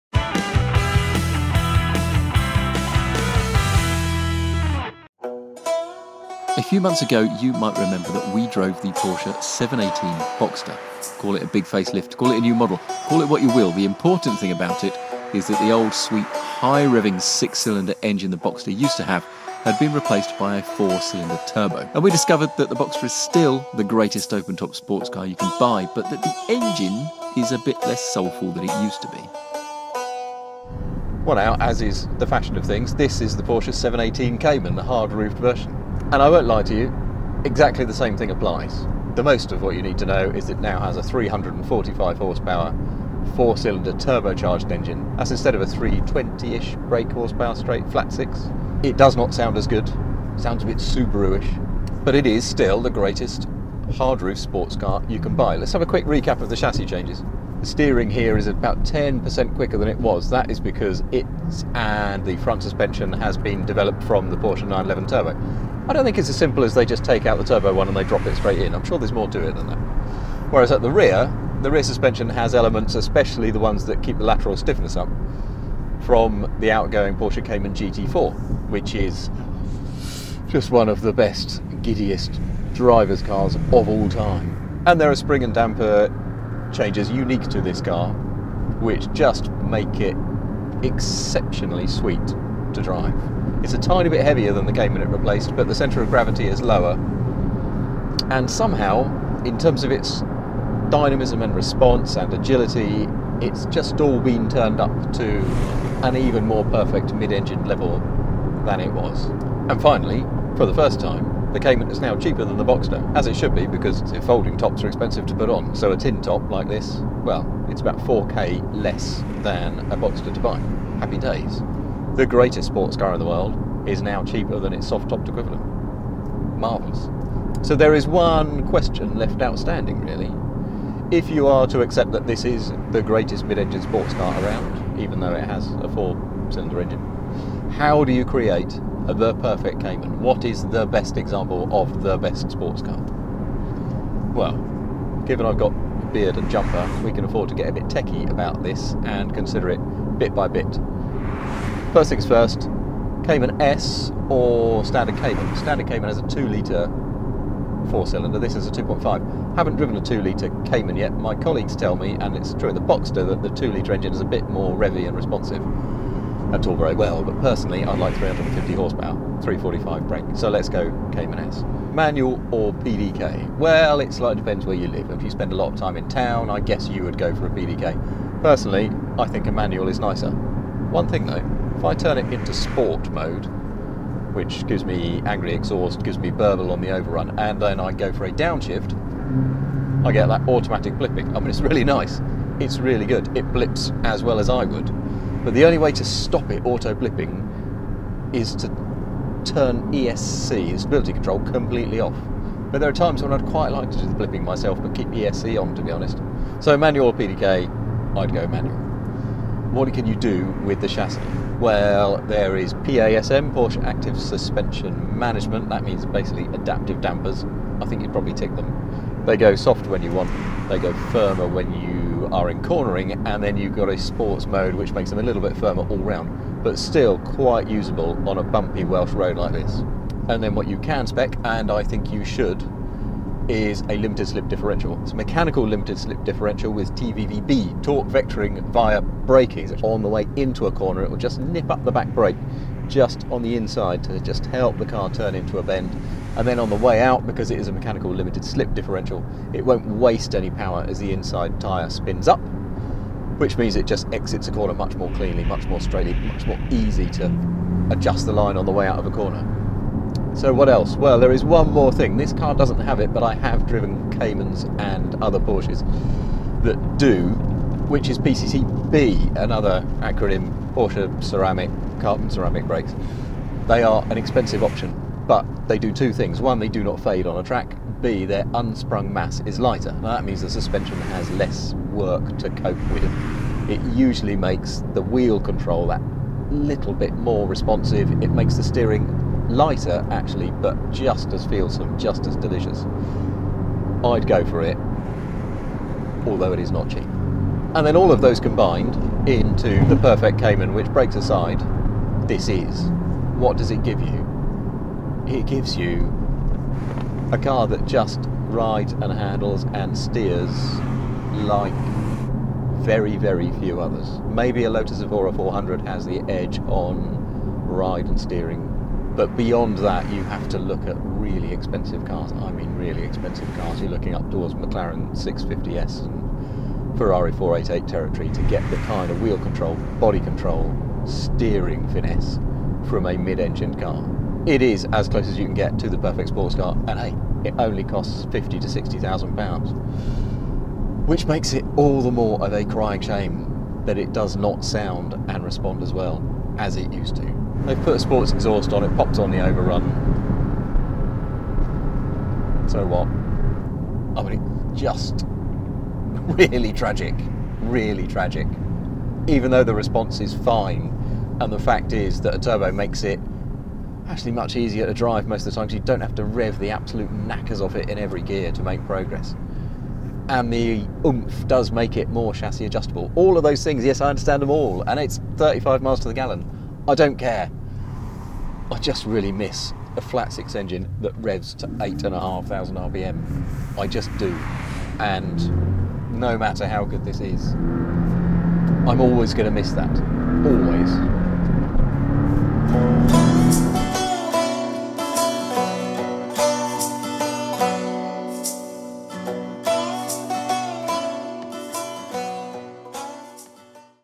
Porsche 718 Cayman S - still the perfect sports car? | First Drive | Autocar
Porsche has launched the 718 Cayman, and we've driven it on the some of the finest roads in the UK.

Like the 718 Boxster we drove a few months ago, Porsche has given the 718 Cayman a four-cylinder turbocharged engine, rather than the old, soulful six-cylinder. Does it matter as much here? Or is the four-pot really a fine replacement?

We find out, and wonder: if the 718 Cayman still is as close as you can get to being the perfect sports car, what from Porsche's huge options list would you need to tick to make it the optimum version of it?

Which Cayman would you have?